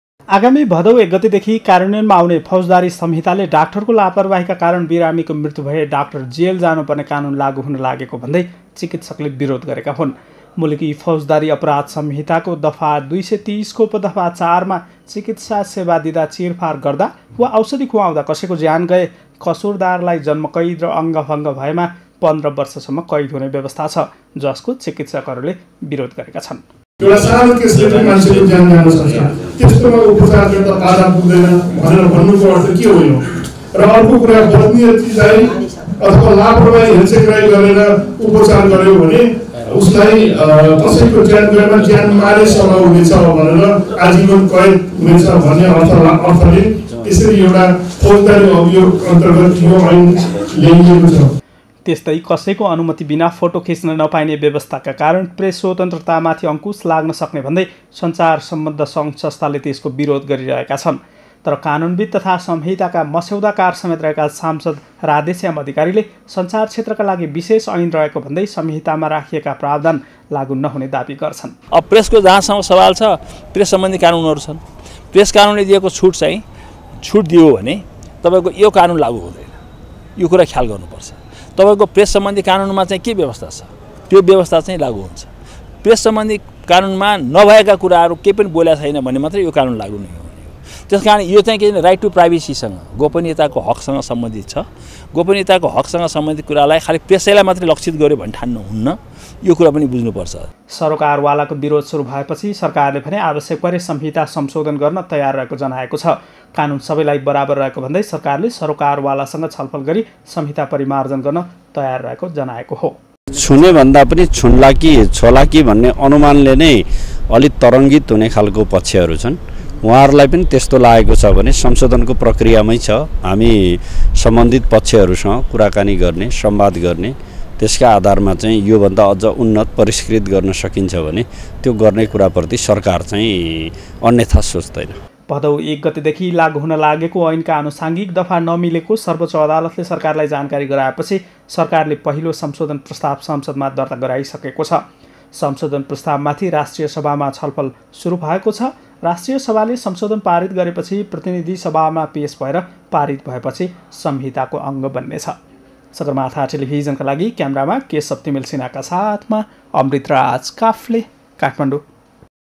ऐन लागु नहुंदै लफडा :
एक सय ६५ वर्ष पुरानो मुलुकी ऐन प्रतिस्थापन हुनेगरी आगामी भदौ १ गतेदेखि कार्यान्वयनमा आउन लागेका संहिता लागु नहुँदै विरोध शुरु भएको छ । संहितामा रहेका केही प्रावधानले चिकित्सकको मनोविज्ञानमा नराम्रो असर पर्ने भन्दै डाक्टरहरुले विरोध शुरु गरेका छन् । केही दफाले प्रेस स्वतन्त्रता समेत कुण्ठित गर्ने भन्दै विरोध भएको छ । सरकारले भने आवश्यक पर्दा ऐन शंसोधन हुनसक्ने भन्दै आएको छ ।